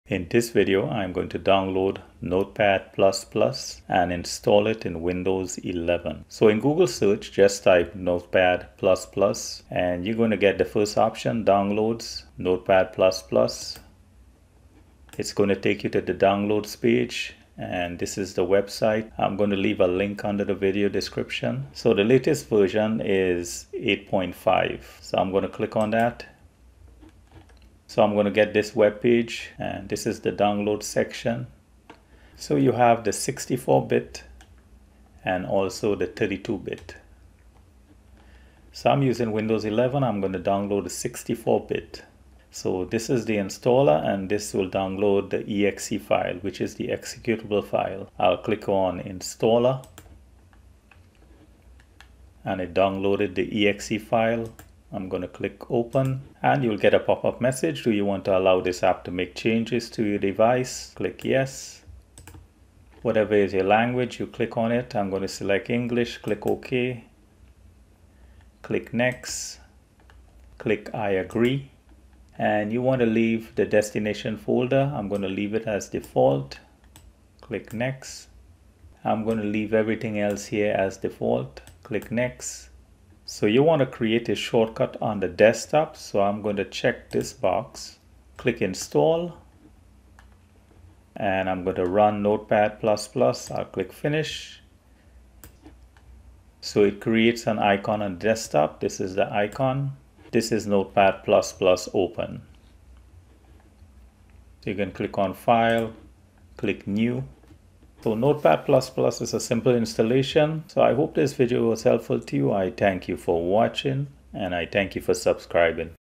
How to install Notepad++ in Windows 11
IN this video, I install Notepad++ in Windows 11. I show you step by step from downloading notepad++ to installing the software.